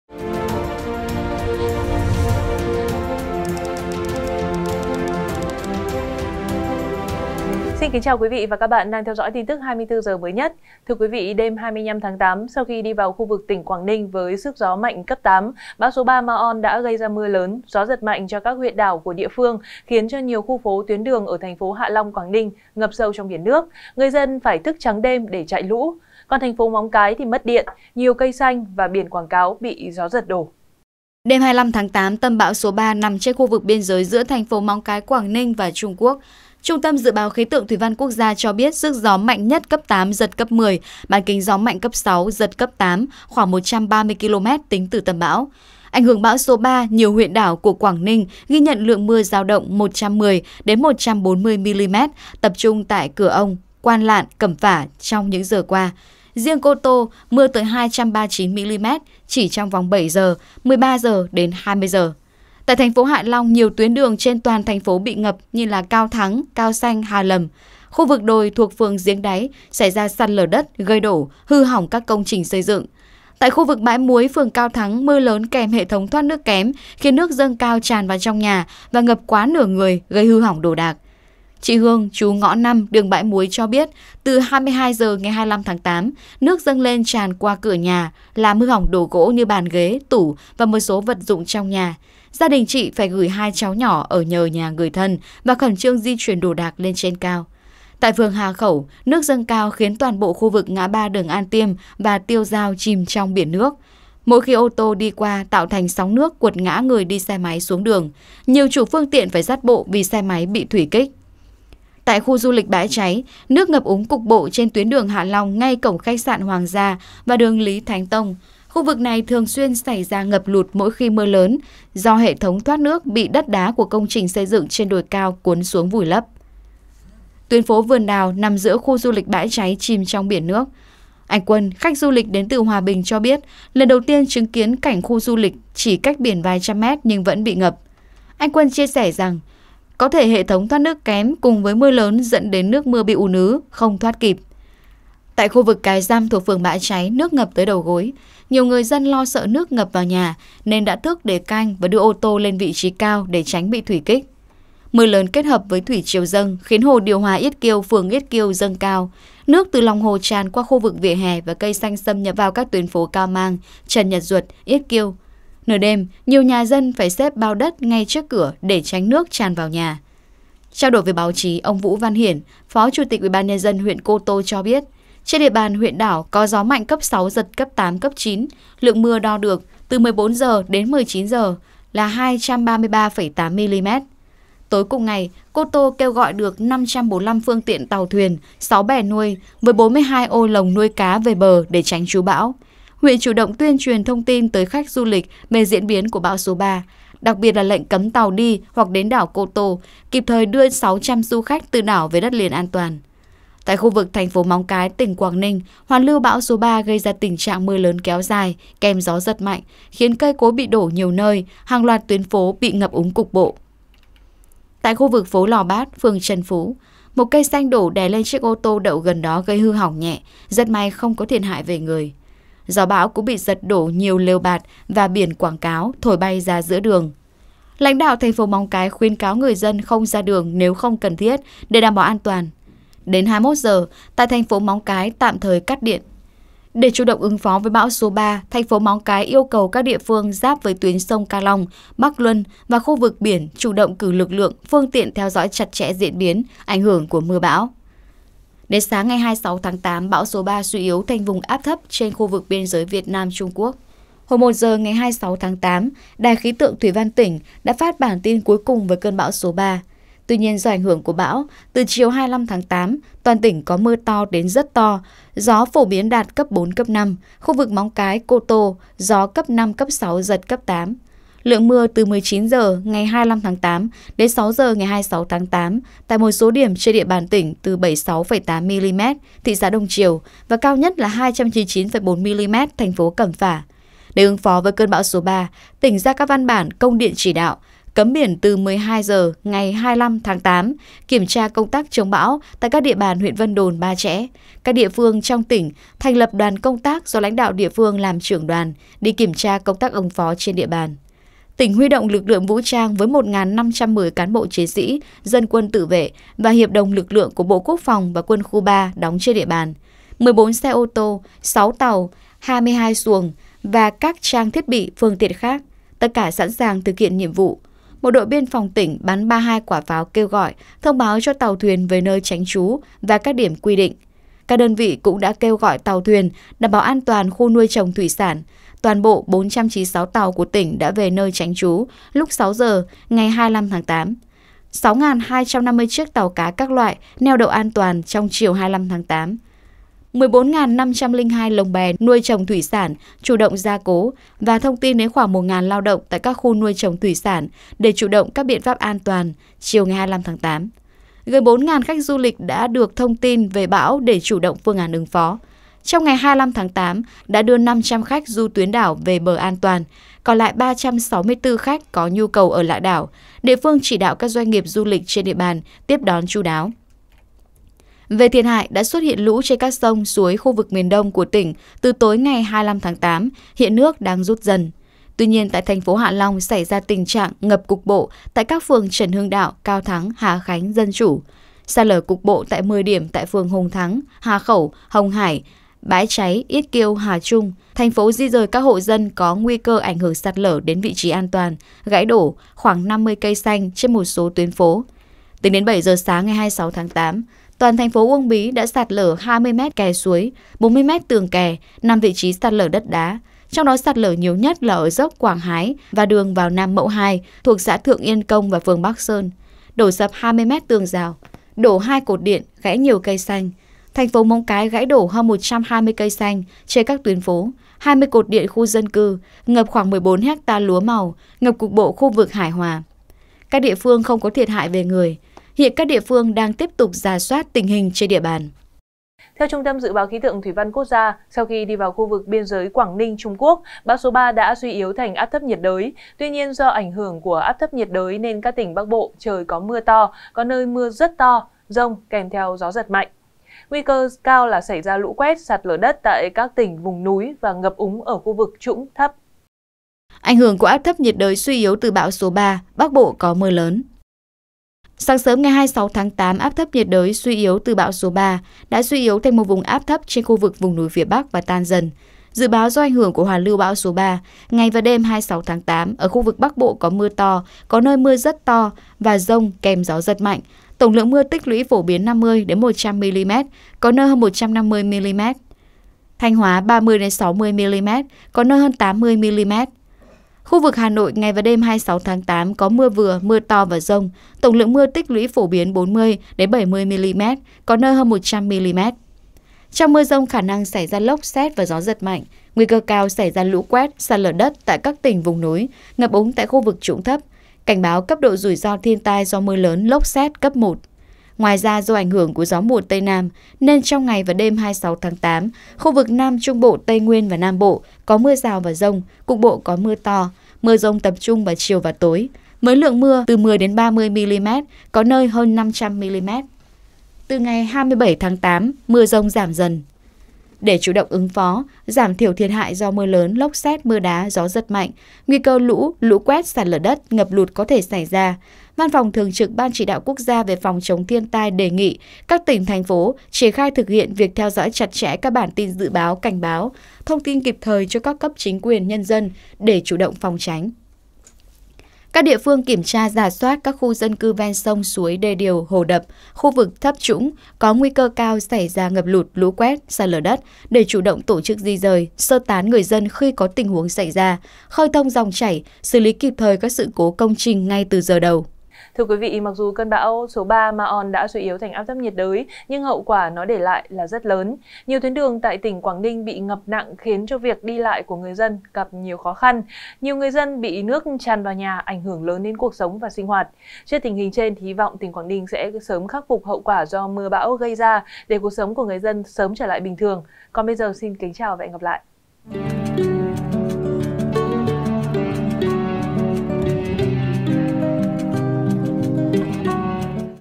Bão Số 3 Vào Quảng Ninh: Mưa Như Trút Quật Cây Gãy Đổ, Người Dân Hạ Long Chạy Lũ Xuyên Đêm | SKĐS
SKĐS | Do ảnh hưởng của cơn bão số 3, mưa lớn kéo dài đêm 25/8 khiến nhiều tuyến đường của các địa phương trong tỉnh Quảng Ninh bị ngập cục bộ, gãy đổ cây cối, sạt lở, nặng nhất là tại thành phố Hạ Long. Nhiều khu phố, tuyến đường ở TP Hạ Long ngập sâu trong biển nước, người dân phải thức trắng đêm để chạy lũ.

Mời quý vị và các bạn xem thêm: Vụ Cán Bộ QLTT Đánh Người Tình Nhập Viện Ở Thanh Hóa: Lộ Tin Nhắn Xin Lỗi, Đổ Lỗi Do Say Rượu